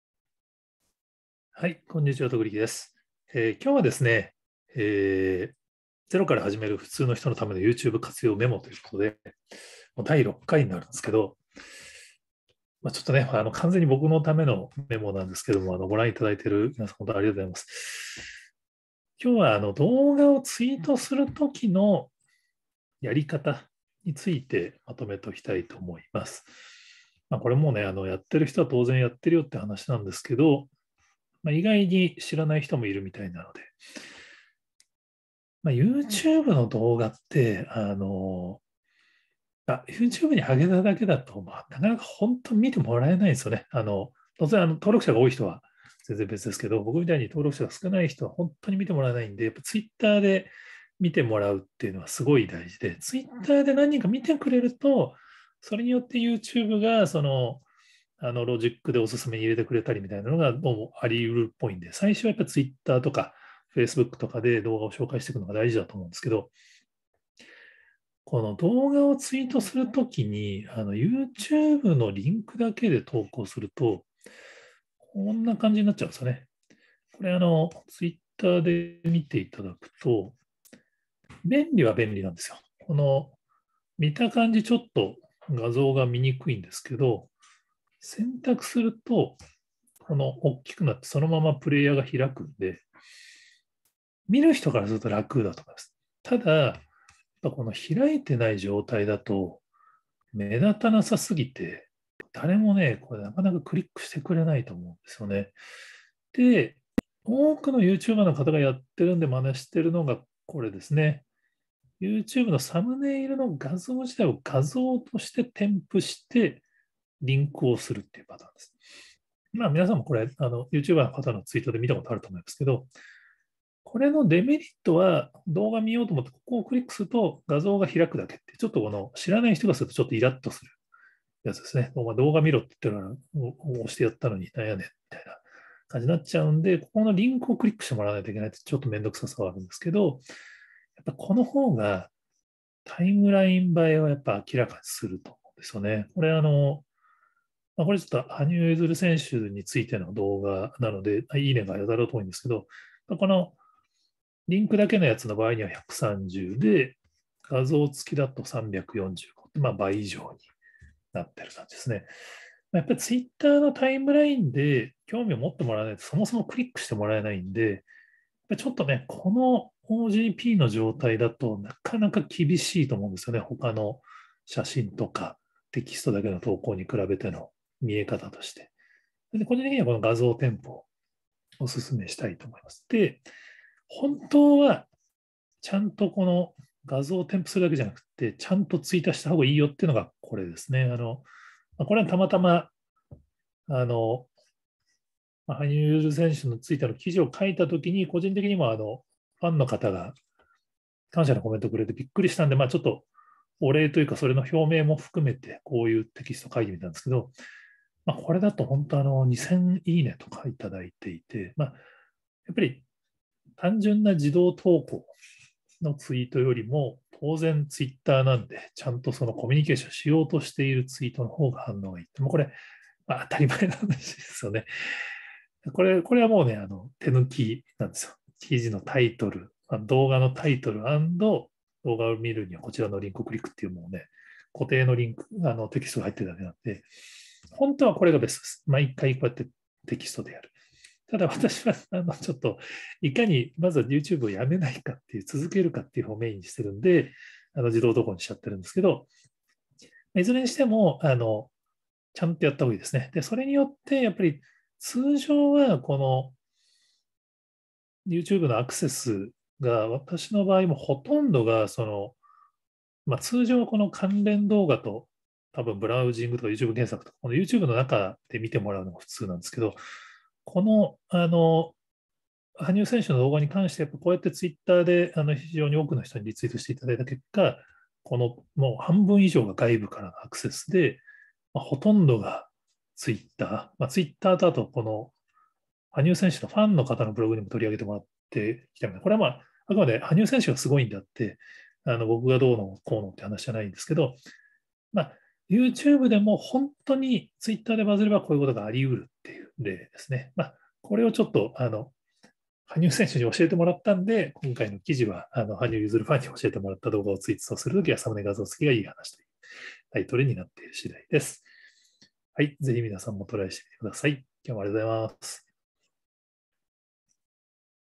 YouTubeの動画をツイートするときは、サムネ画像付きが良さそう
「未来を楽しく想像する」をテーマに、自分が書いた記事の裏話をご紹介してます。
良ければ記事もご覧ください。

■羽生結弦ファンに教えてもらった、動画をツイートするときはサムネ画像付きが良い話